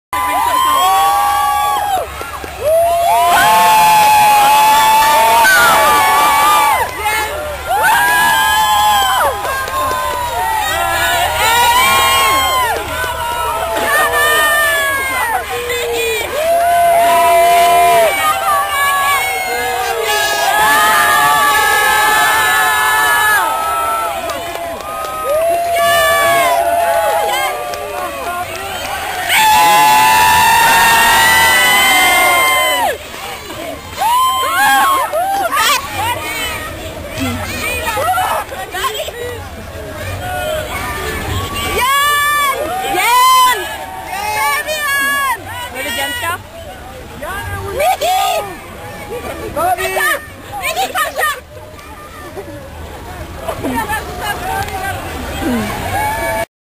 Saxo Bank at the Maison du Danemark 2010
Team Saxo Bank at the Maison du Danemark on the Champs-Élysées in Paris during the lap of honour of the 2010 Tour de France.